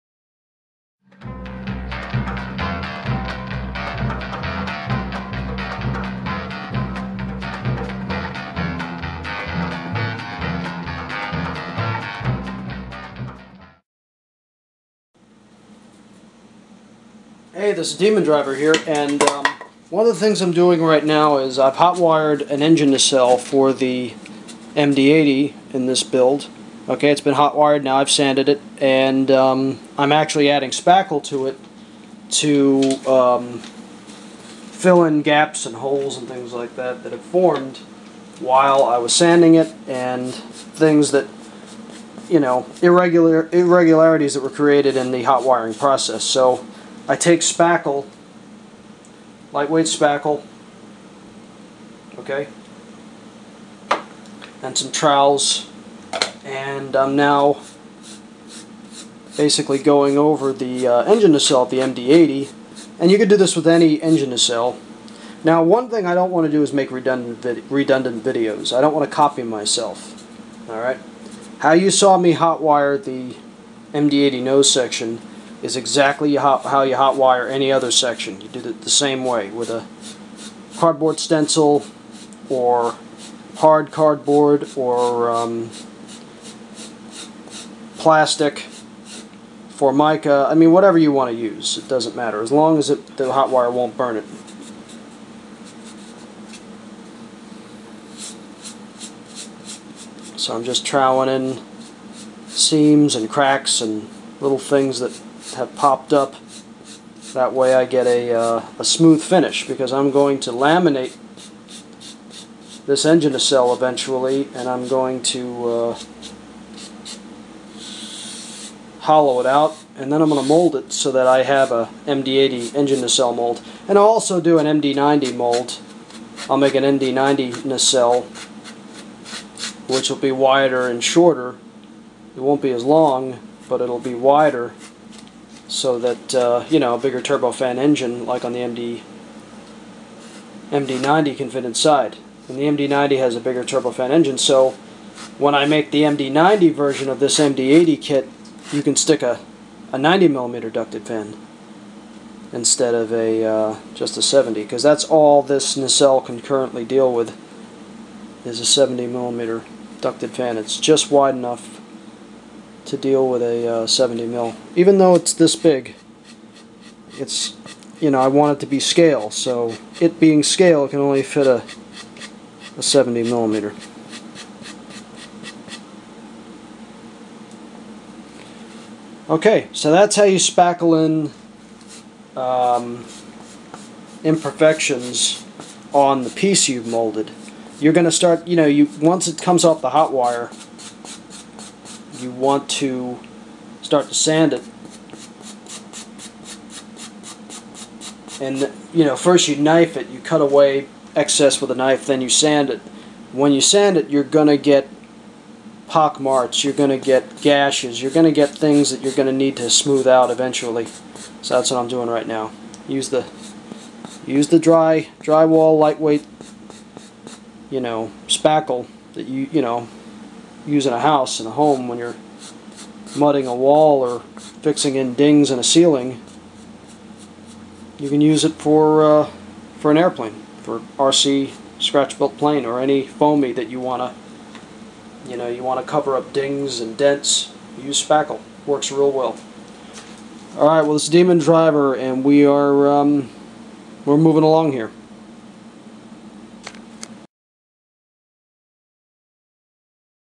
How to build a 7.4 Foot MD-80 RC EDF Airliner Part-16!
Hi this is Demondriver from RCgroups.com & I'm gonna share with you my 7.4 foot long Mcdonnell douglas MD-80 I'm building seven actually 2 to be sold on EBAY and soon to be tuned into a hybrid fiberglass depron kit, Specs lenght 88inches wingspan 68 inches, material blue fan-fold foam, depron, carbon fiber, balsa wood, ply-wood, pink corning foam & more! Enjoy!